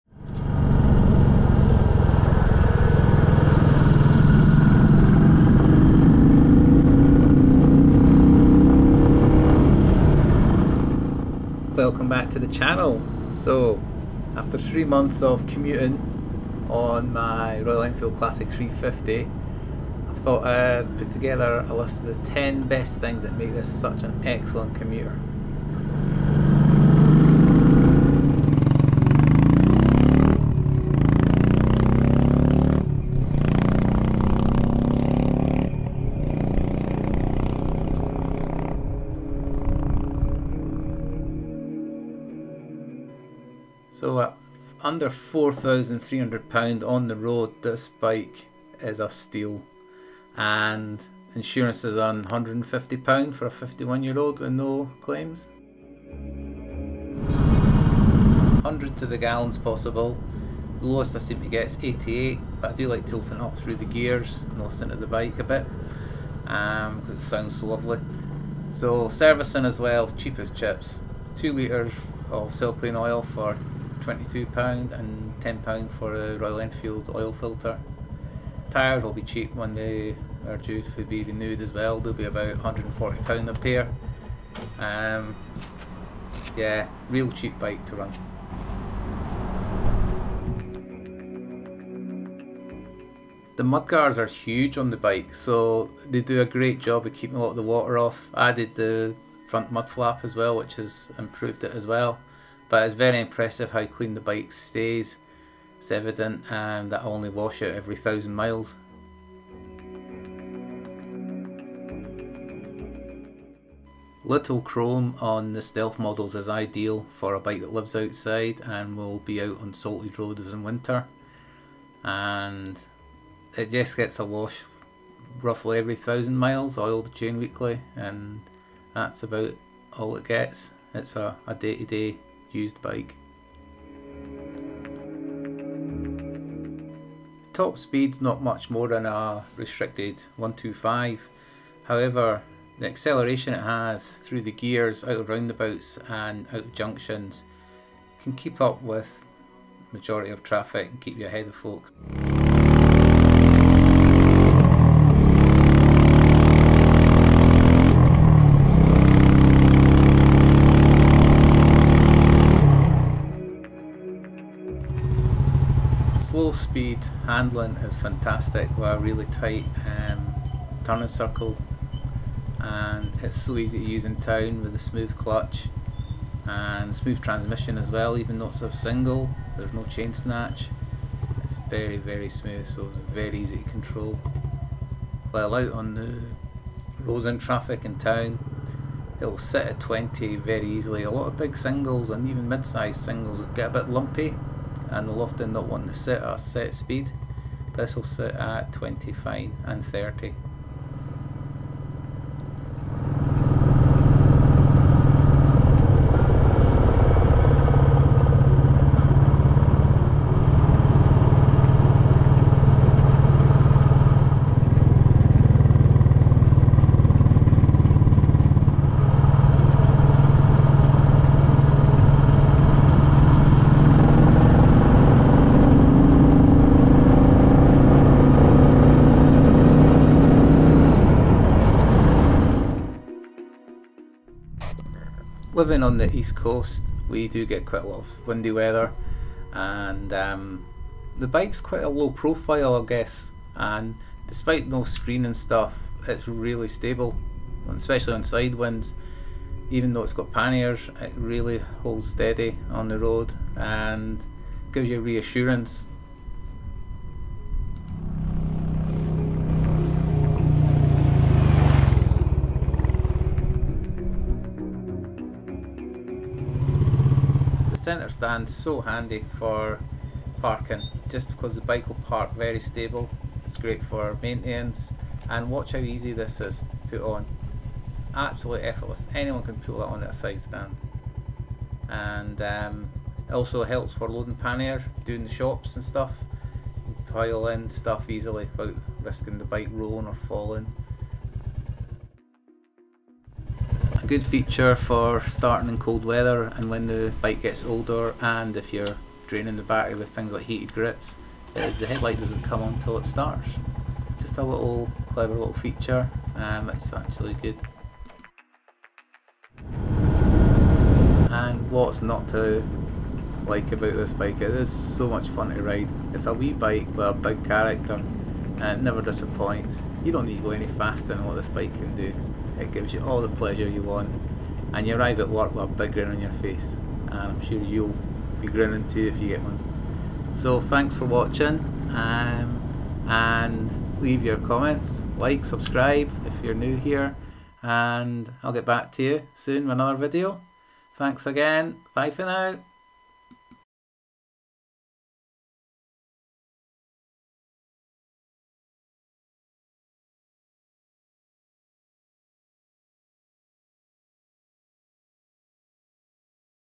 Royal Enfield Classic 350 - 10 Reasons the Royal Enfield Classic 350 excels for Commuting
10 things I appreciate about the Royal Enfield Classic 350 after 3 months commuting and use as my daily transport